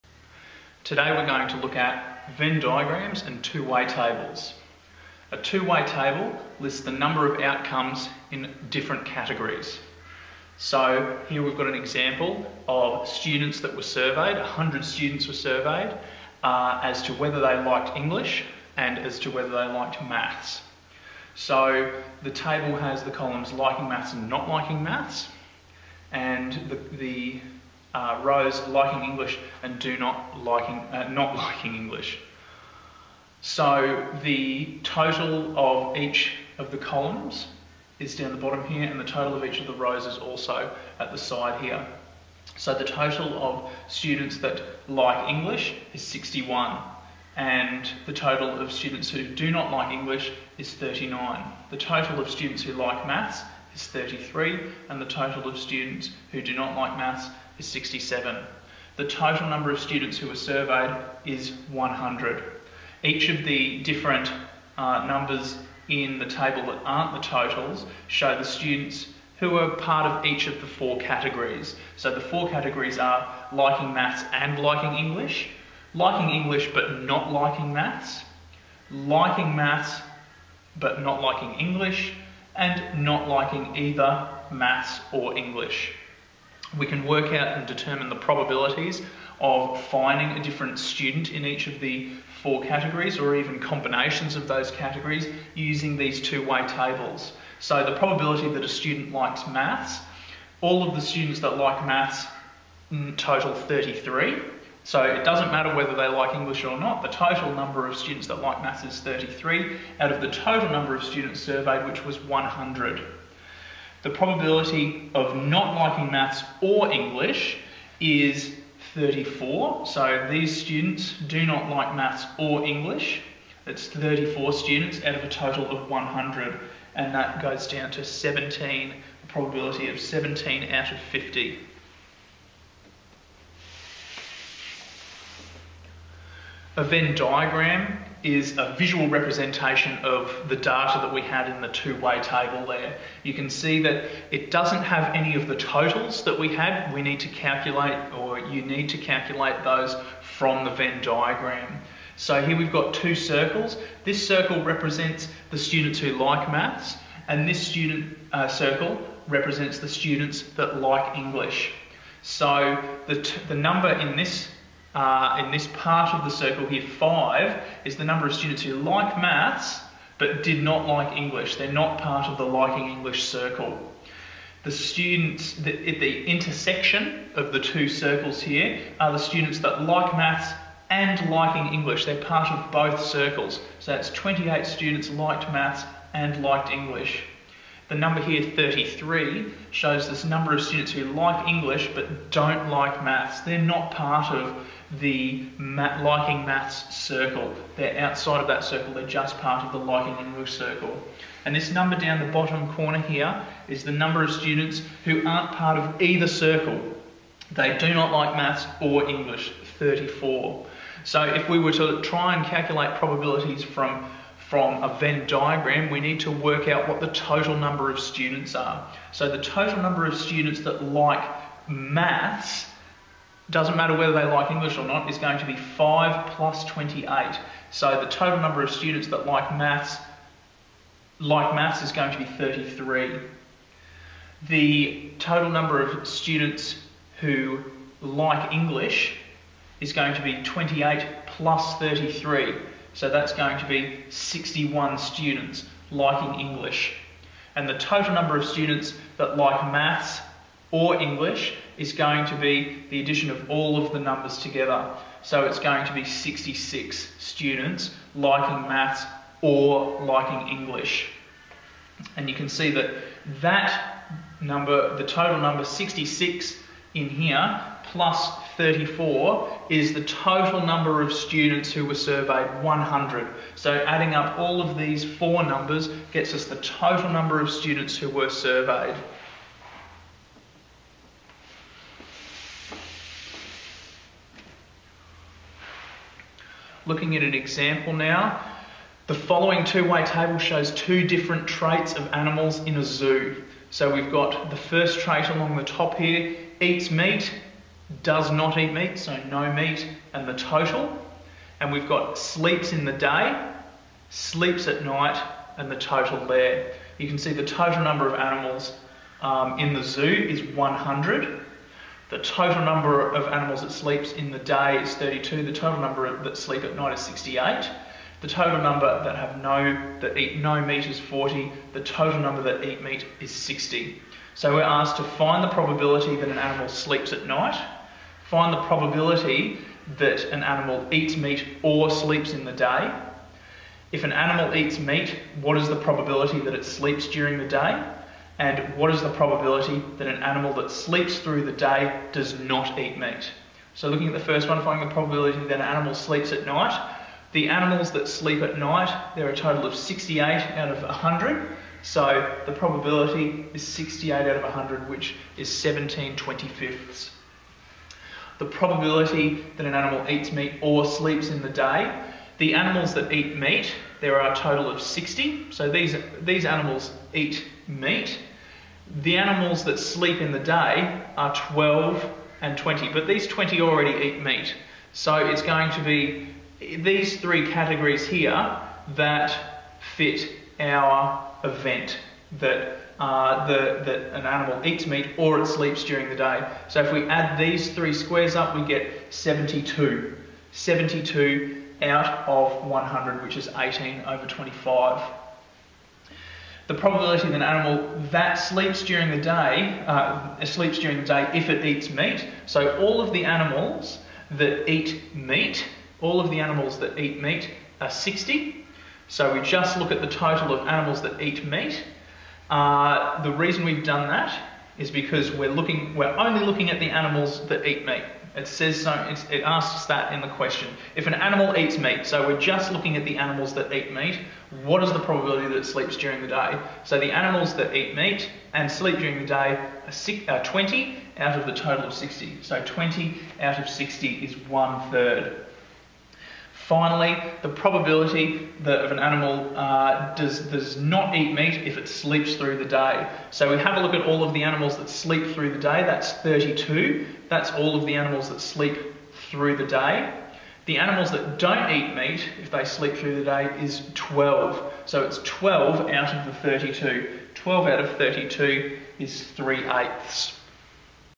Venn Diagrams and Two-Way Tables
Students learn how to interpret Venn Diagrams and Two-Way Tables and use them to calculate probability